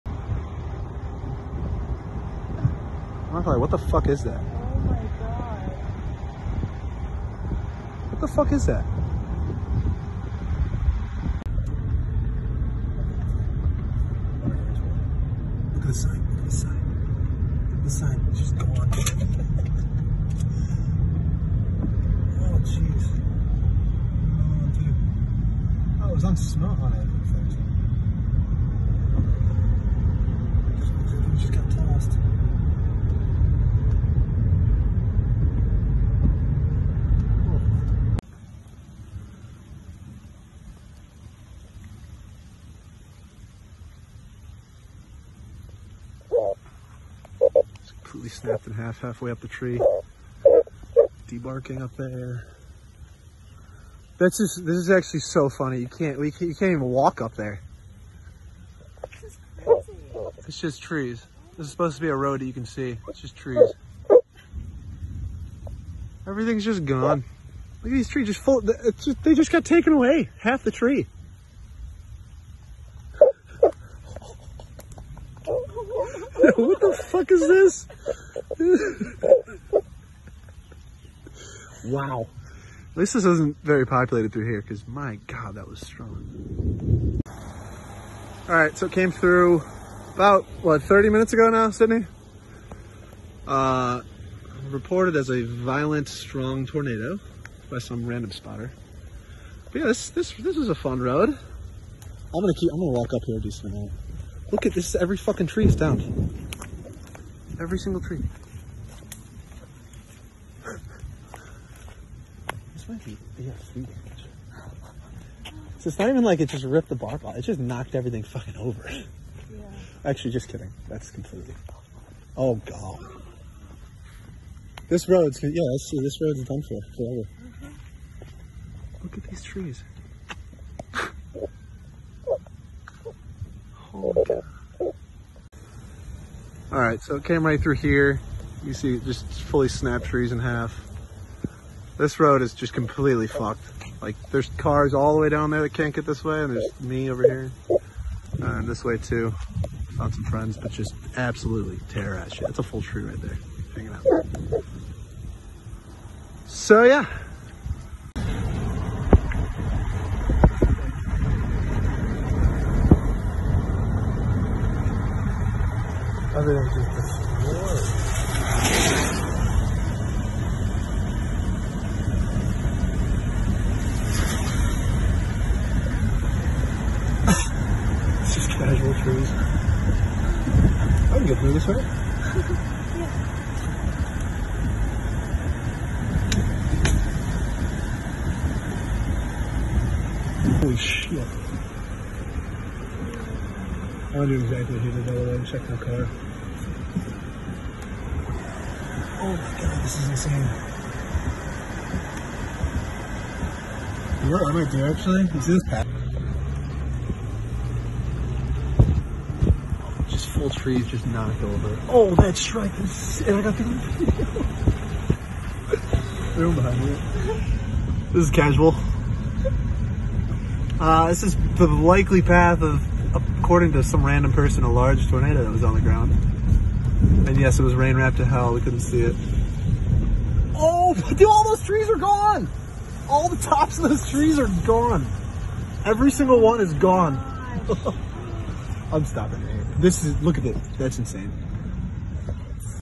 Tomah, Wisconsin Tornado and Tornado Damage - June , 15, 2022
A brief video of the Tomah, Wisconsin Tornado from June 15, 2022, followed by video clips of the blown-over semis on I-94 and then the tree damage that occurred from the tornado.